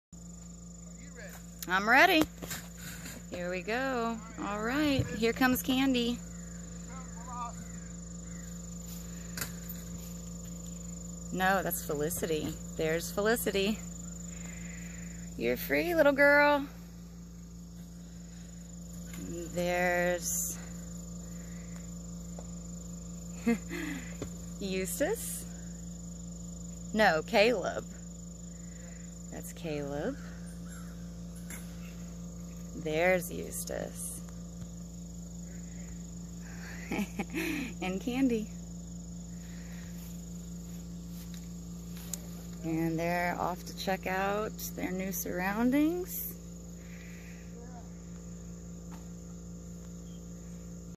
SECOND RELEASE!
From left to right, Caleb, Eustice, Candy and Felicity are released at the farm! Off to your new lives in the wild. It was certainly an honor to help them!